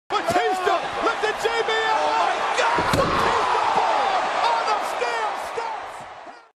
PowerBomb On The Steel Steps! (WWE Figure Stop Motion)
A Huge powerbomb onto the steel steps from JXT to Syd Parker getting ready for the 3rd match in their best of 7 series!
Who do you think will win the series??